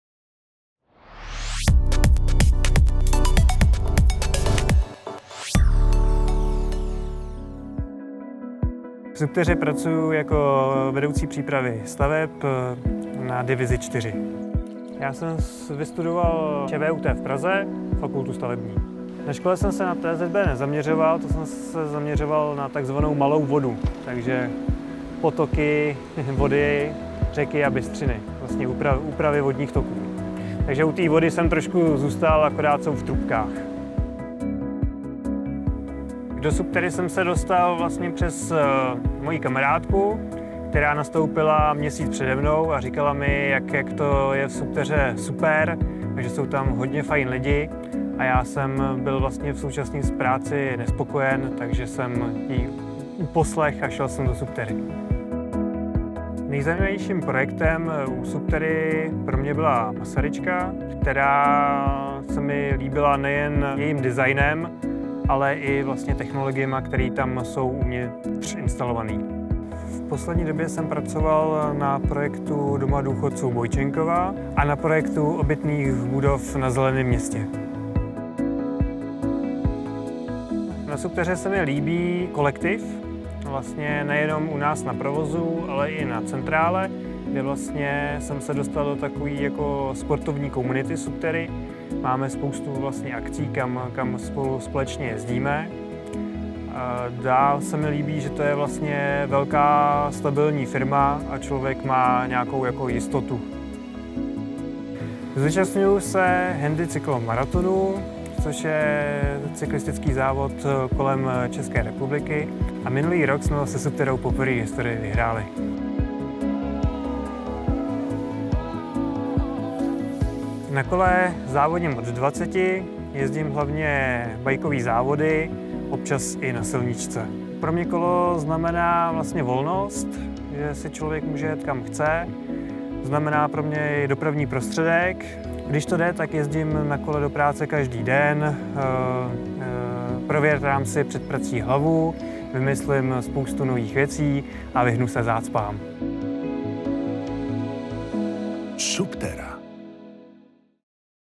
SUBTERRA – Naši lidé – Václav Šuser
Václav Šuser přišel do společnosti Subterra na doporučení kamarádky a už u nás zůstal. Kromě toho, že pracuje jako vedoucí přípravy staveb na divizi 4, tak stíhá i úspěšnou závodní kariéru bikera.

Na jakých projektech dělá a co na práci v naší společnosti oceňuje?

Chcete s námi měnit svět kolem? Běžte na a zjistěte, co Vám můžeme nabídnout.